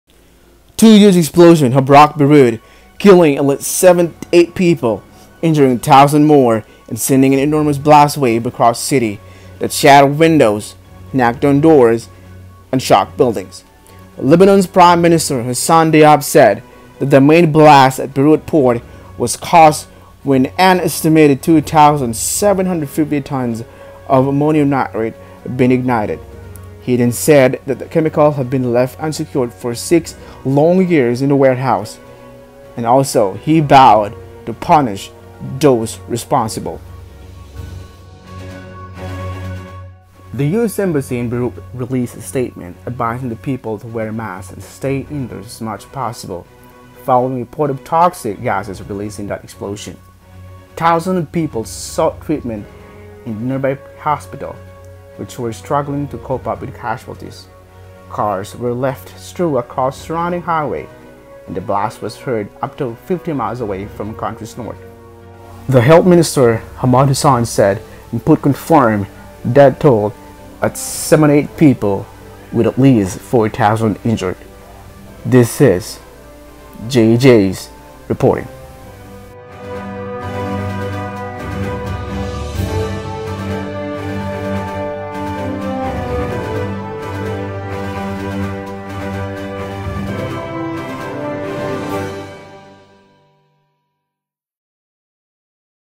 Beirut Lebanon Explosion/Breaking News /Explosion
Two huge explosions have rocked Beirut, killing at least 78 people , injuring thousand more and sending  an enormous blast wave across the city that shattered windows.

 The explosion caused when an estimated 2,750 tons of ammonium nitrate had been ignited .

Credit to theguardian for the news

JeJap TV